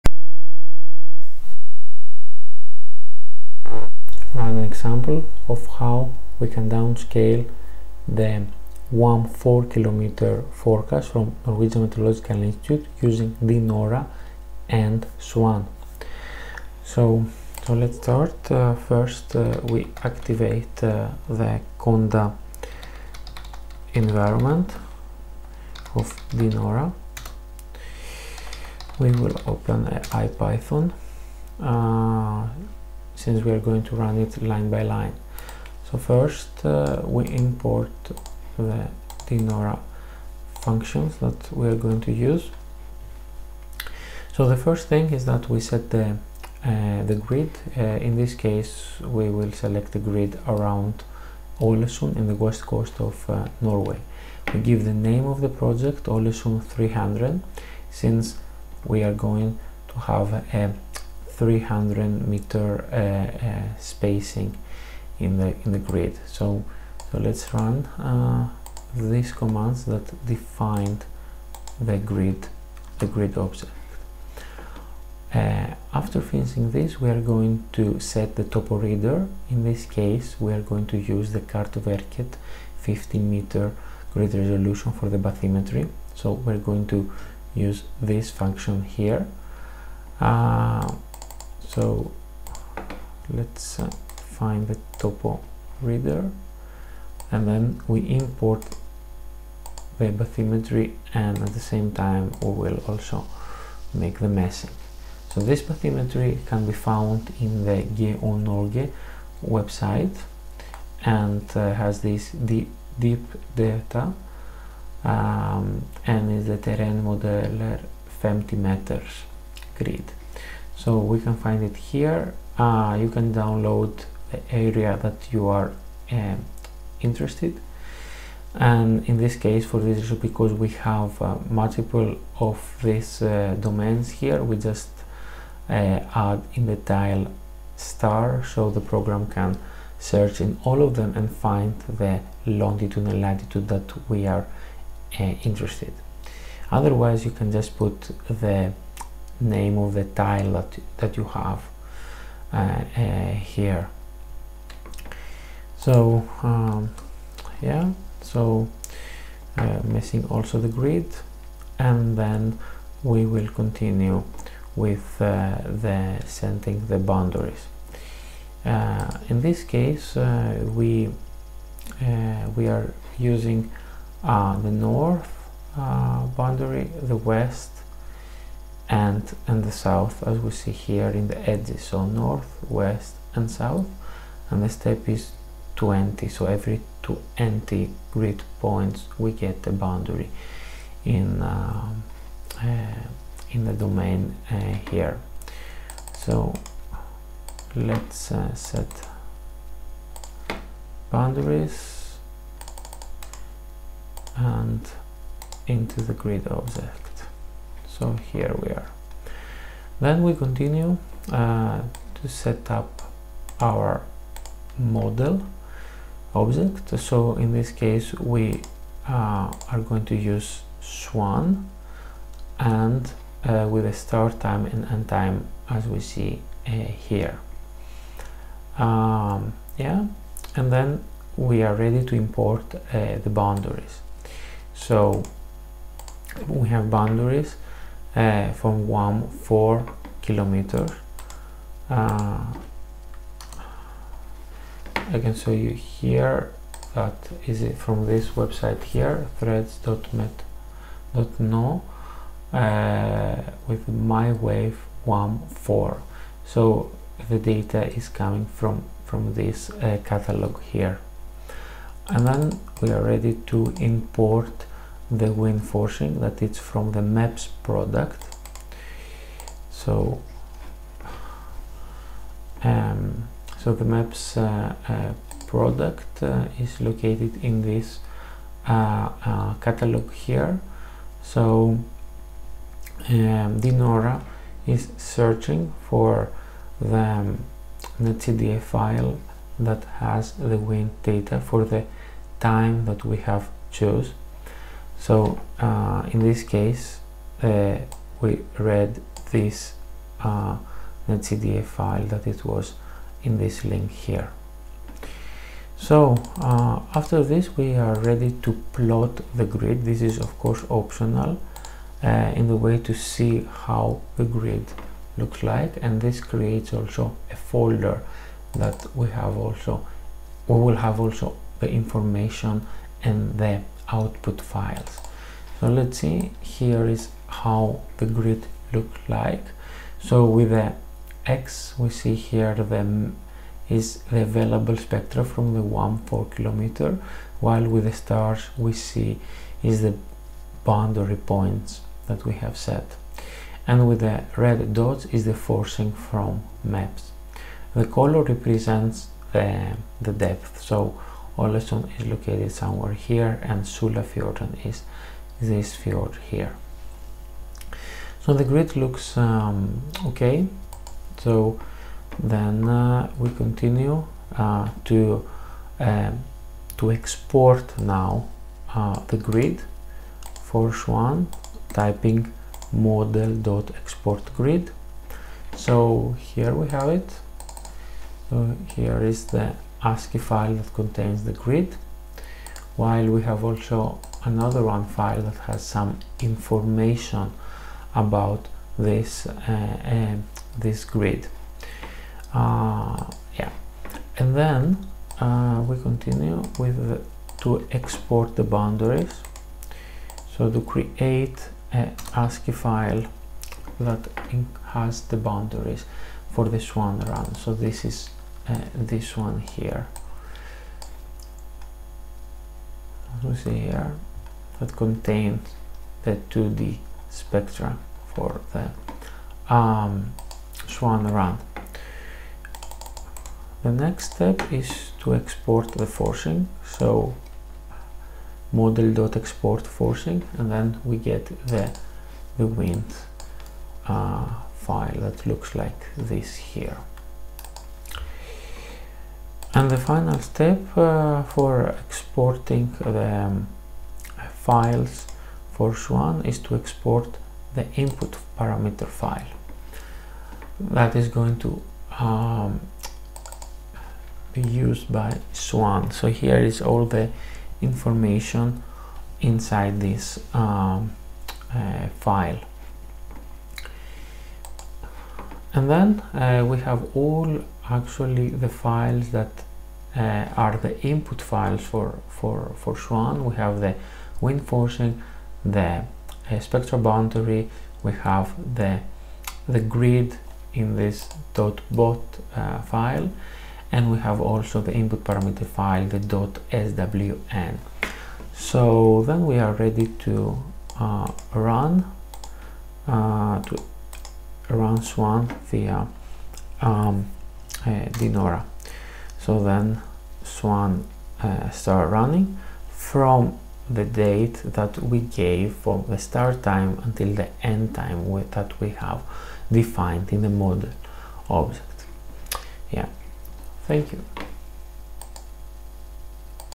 Downscaling WAM4km using DNORA/SWAN
DNORA is a ope-access Python package for dynamical downscaling of MET Norway wave hindcast/forecast products using different wave models.